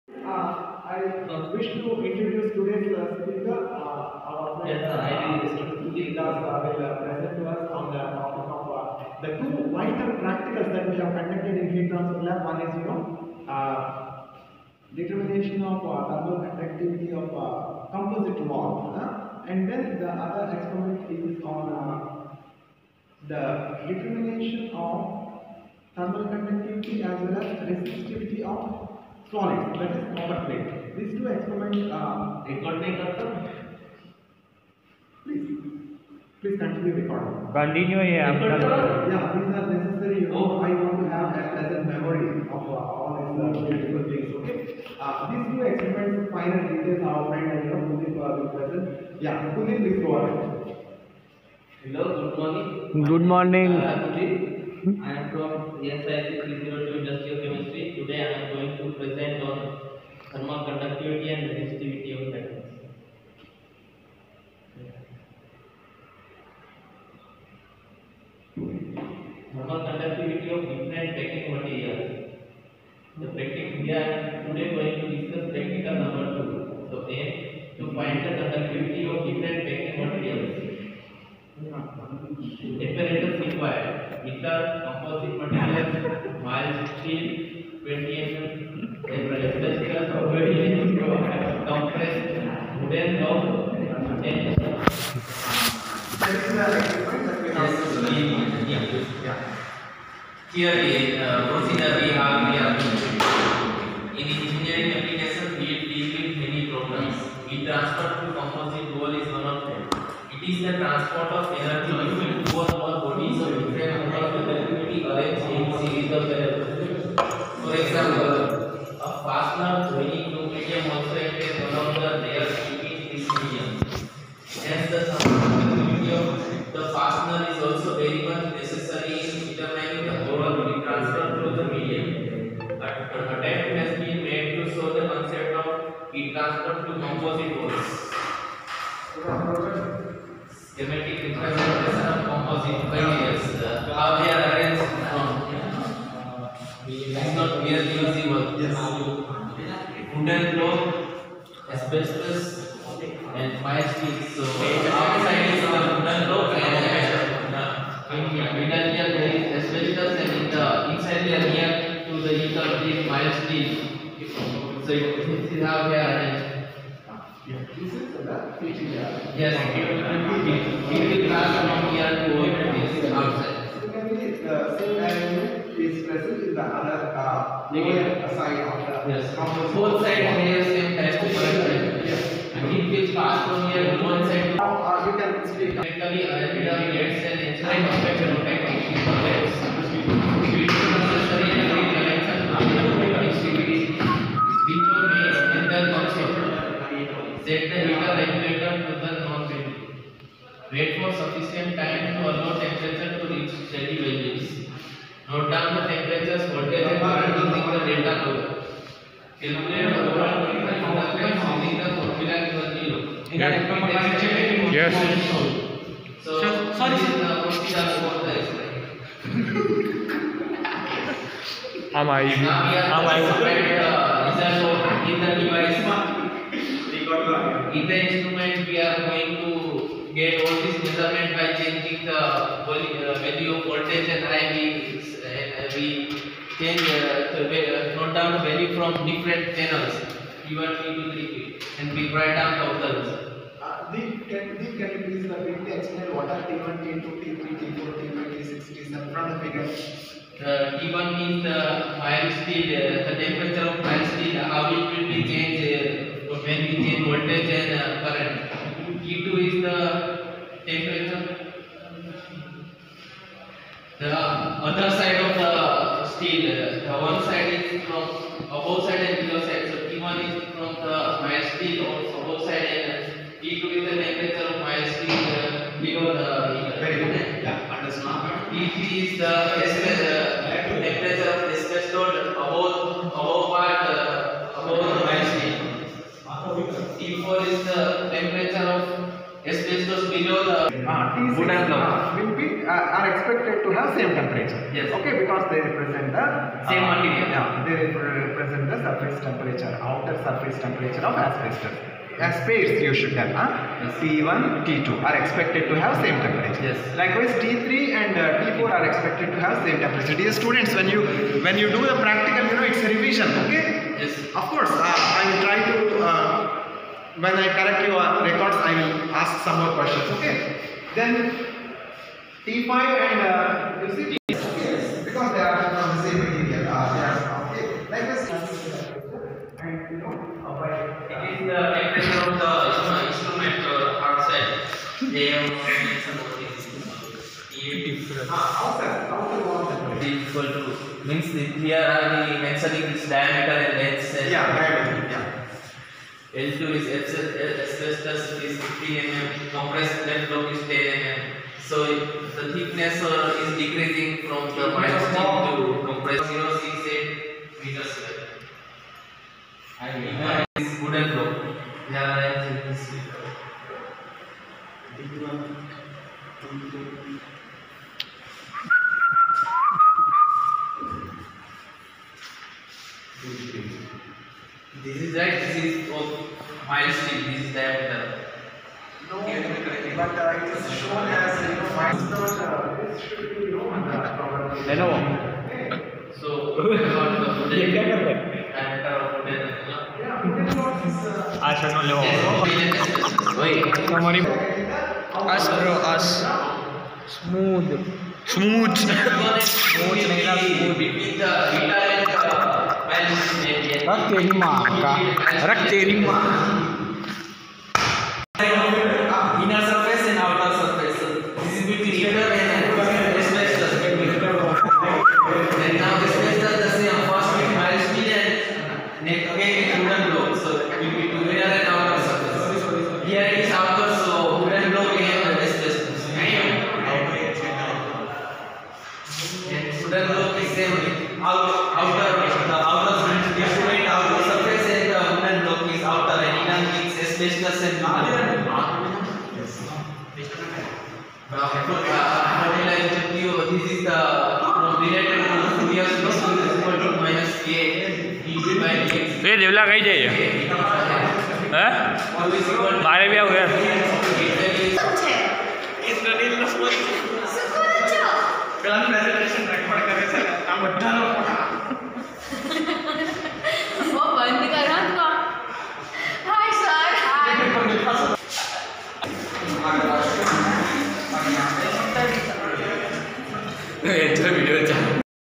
video 20221230 105048_Professor Tae Hyun Kim’s Lecture series 2023
Mr Kuldeep dasa_thermal conductivity and thermal resistivity of metal _Friday
30/12/2022